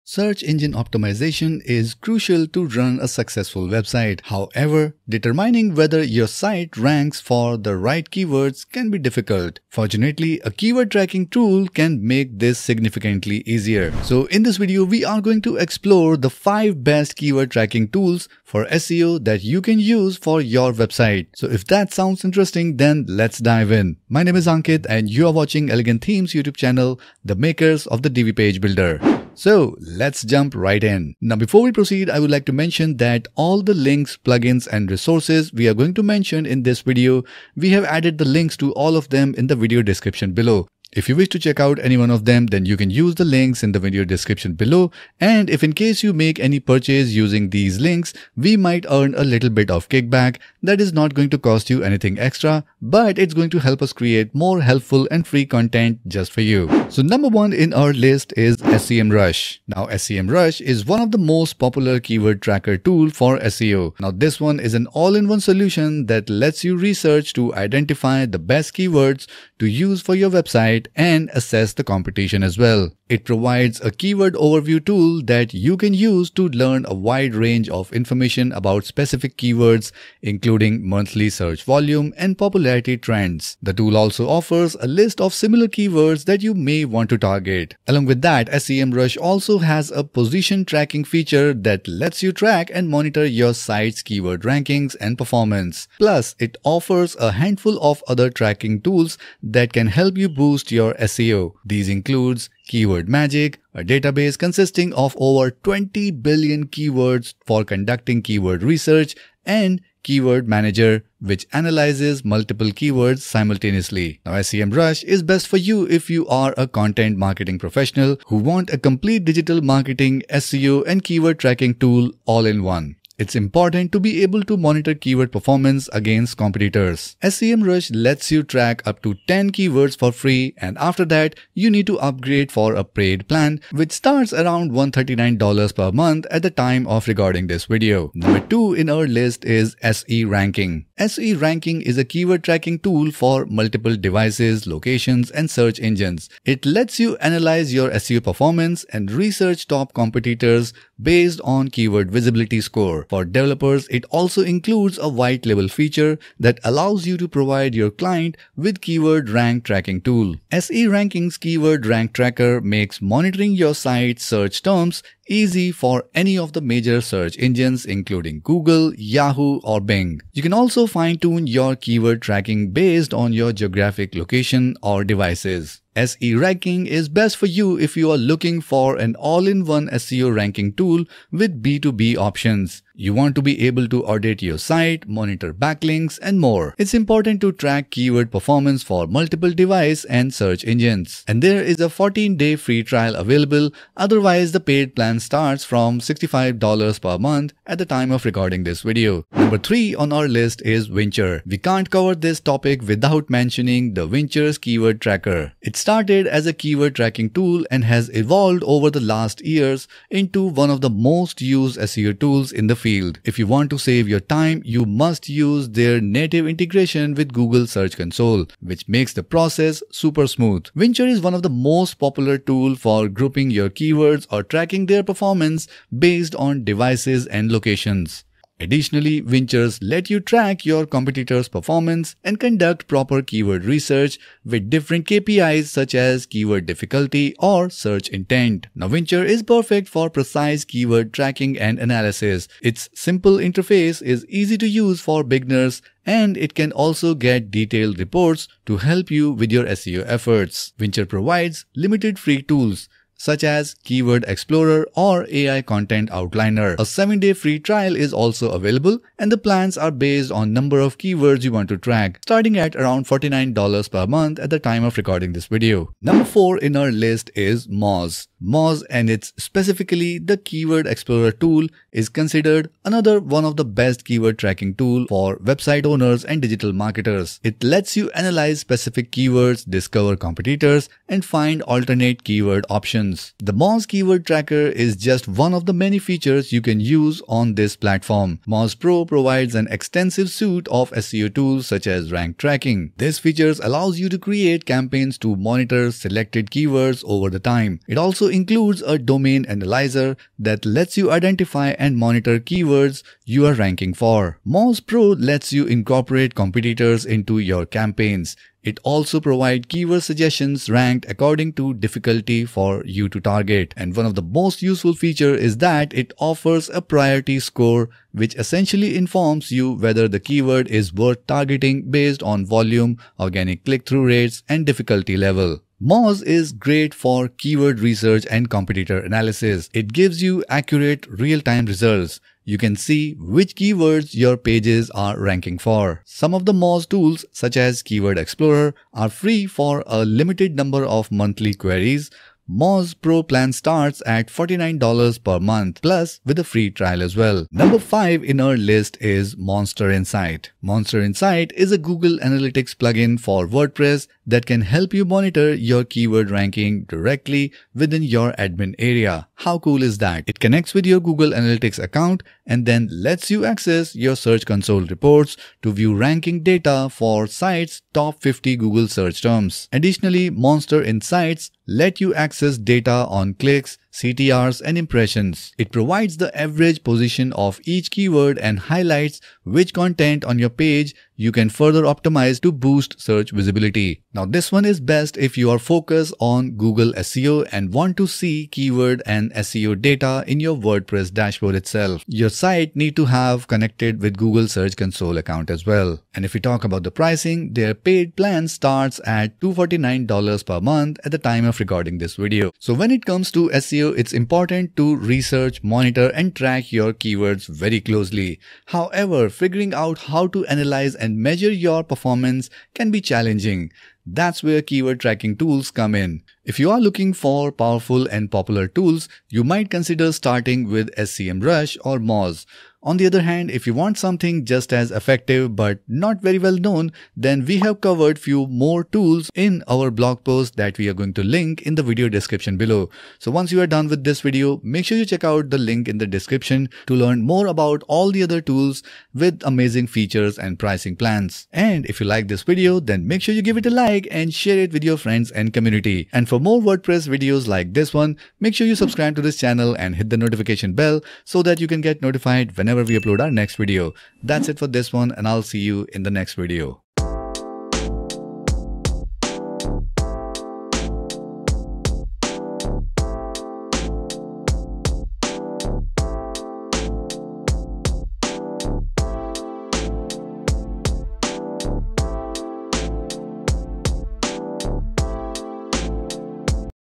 7 Best Keyword Tracking Tools for SEO
Tracking the right keywords is essential for improving your SEO strategy. In this video, we compare the 7 best keyword tracking tools available for SEO in 2024. From advanced analytics to user-friendly interfaces, we’ll break down the features, pricing, and benefits of each tool. Whether you’re a beginner or a seasoned SEO pro, these tools will help you monitor rankings, optimize your content, and boost your search visibility. Let’s dive in!

Timestamps ⏰
00:00 - Introduction
00:31 - Resources
00:57 - Tool No.1
02:18 - Tool No.2
03:19 - Tool No.3
04:26 - Tool No.4
05:48 - Tool No.5
06:43 - Final Thoughts